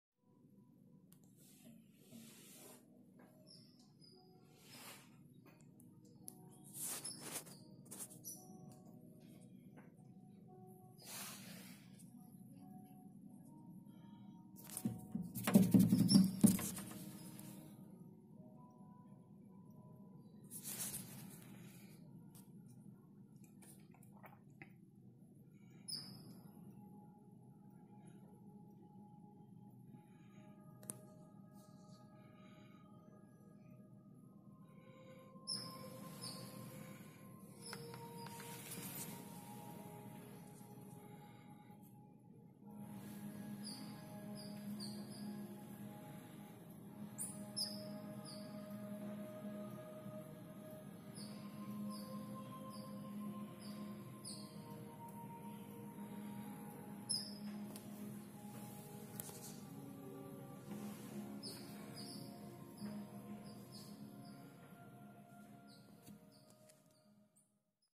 SNAKE 🐍
This is a Rat Snake outside of my house 🏡